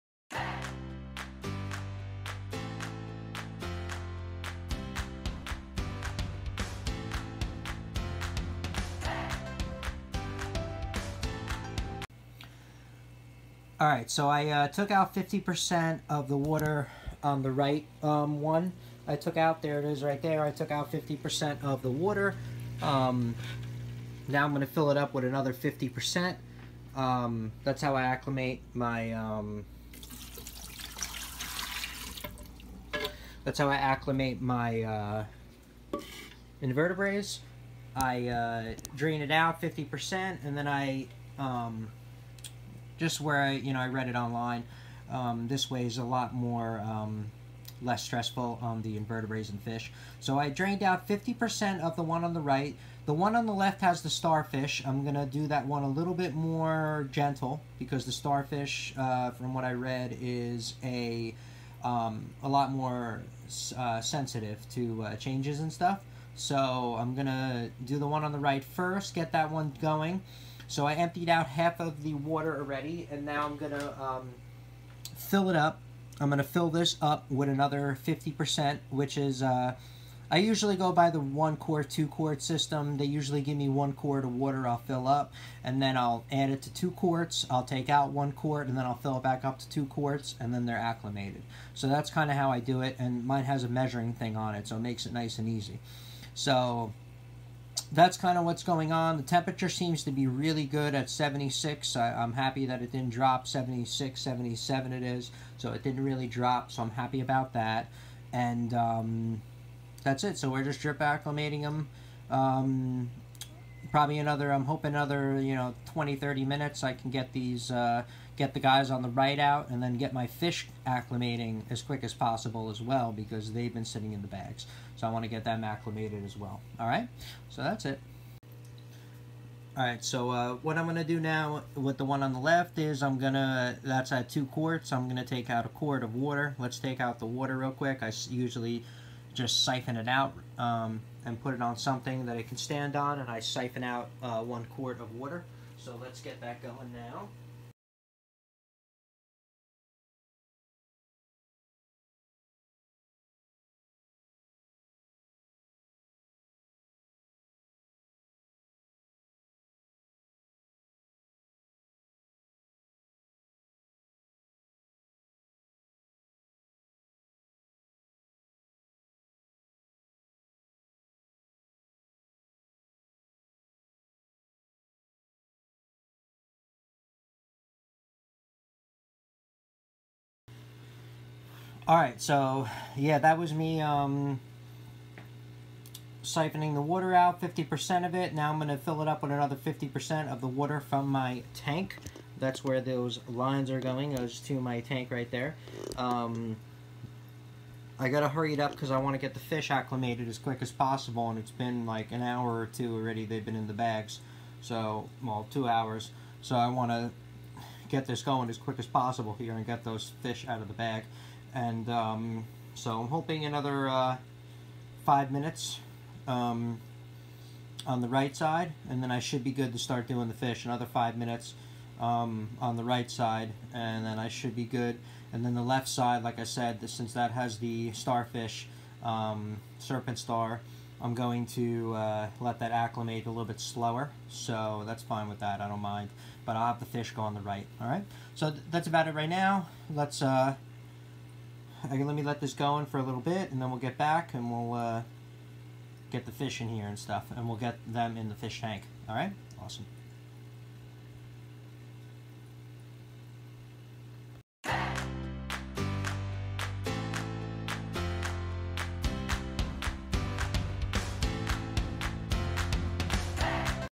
Acclimating my Blue zoo aquatics order part 2.
I know I take awhile to get my videos on. I am just usually busy with work stuff. So this is just an update on what I was doing. Any questions or comments please post them down below. "Music